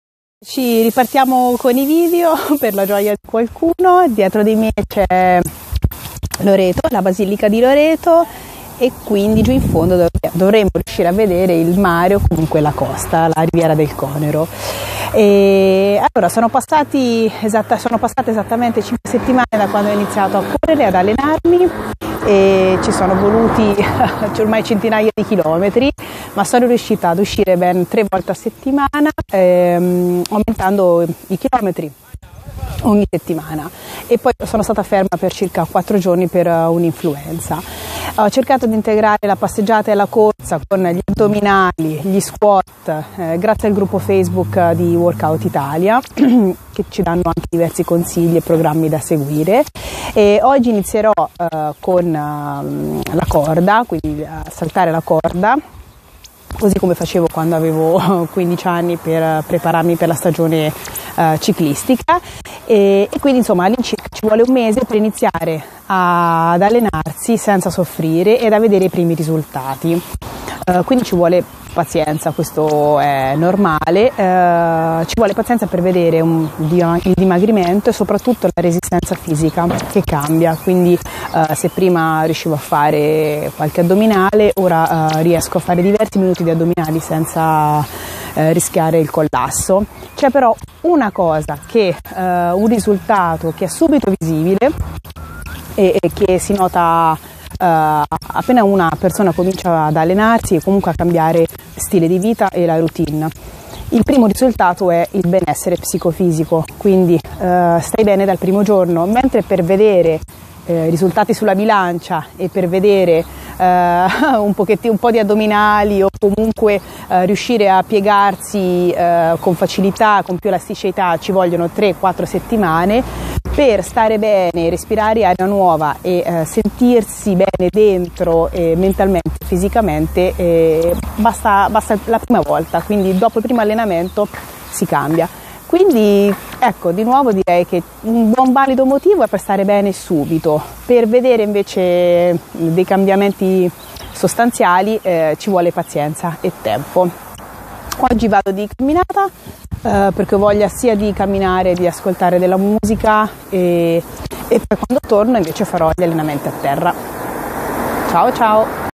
Quando arrivano i primi risultati?
Per alcuni risultati dobbiamo aspettare qualche settimana, ma un risultato è visibile sin dal primo allenamento.